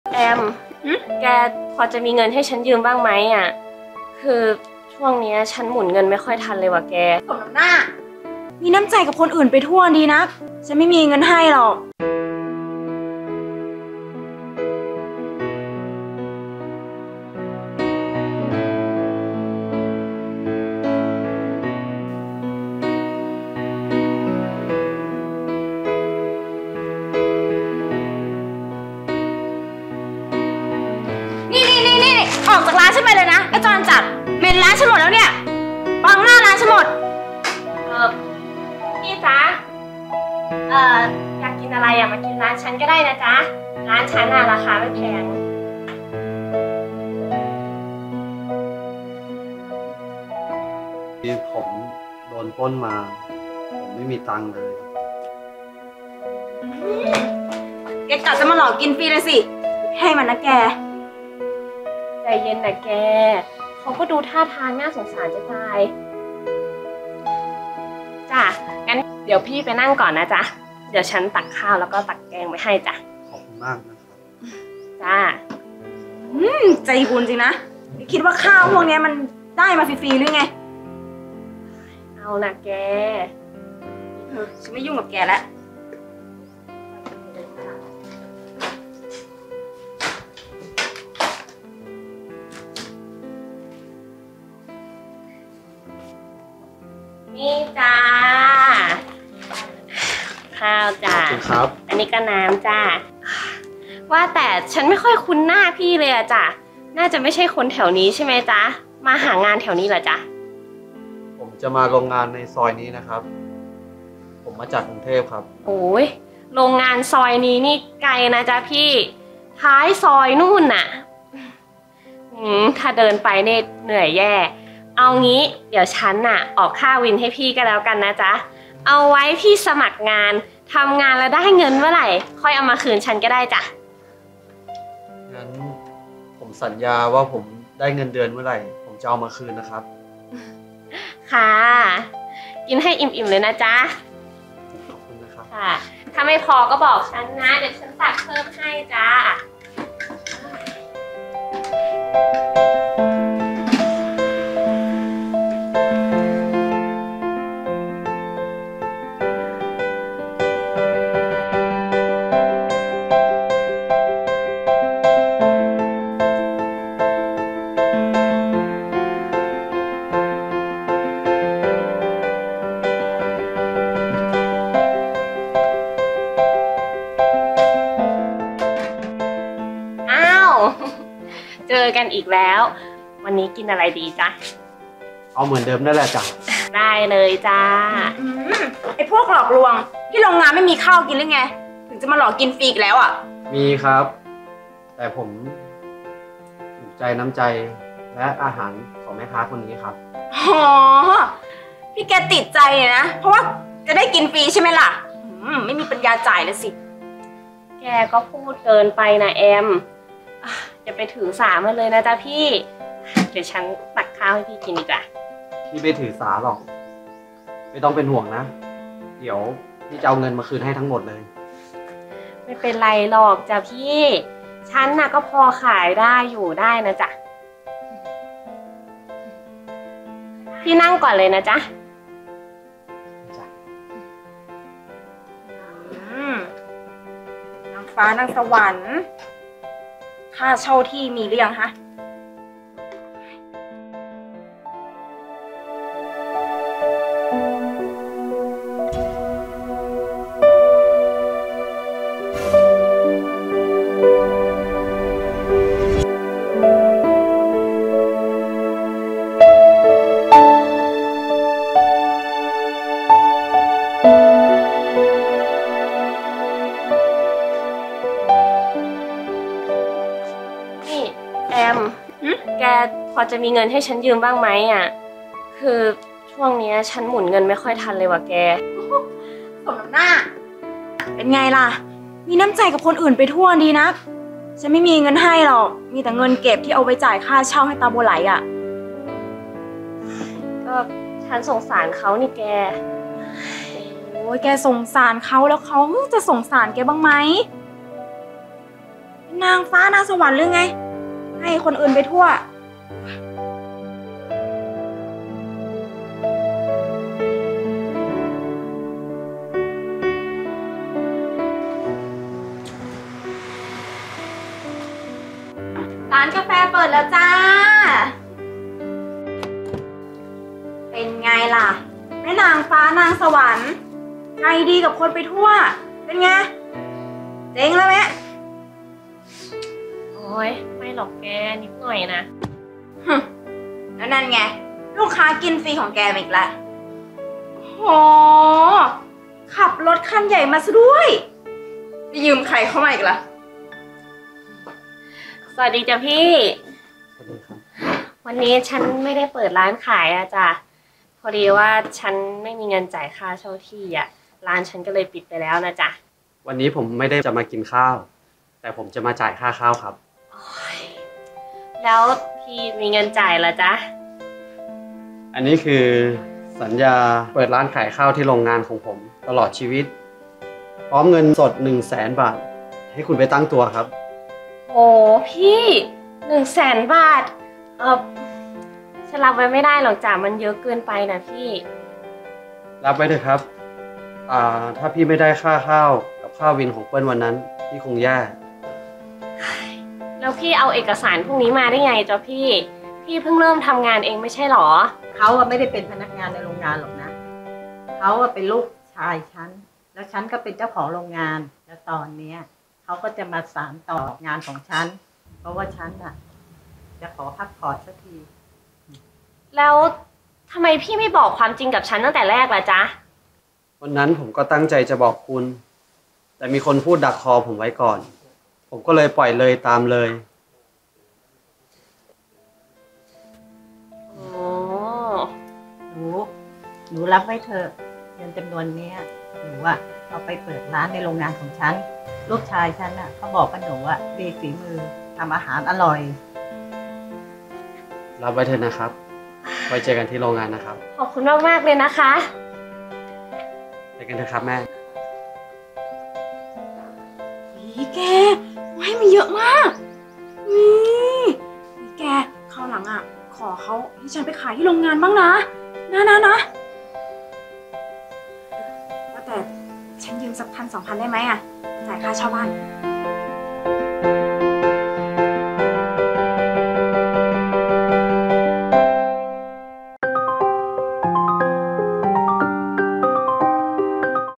คนตกอับที่คุณช่วยเป็นลูกเจ้าของโรงงาน ตาทิพย์ TATHIPHY
คลิปนี้เป็นการแสดงเท่านั้น  โปรดใช้วิจารณญาณในการรับชม รับชมเพื่อความบันเทิง
ละครสะท้อนสังคม ละครสั้น ตาทิพย์ ละครสั้นสะท้อนสังคมที่หลากหลายในปัจจุบันทำให้เห็นมุมมองอีกด้านทั้งดีและไม่ดีที่มีอยู่อาจจะใกล้ตัวคุณก็ได้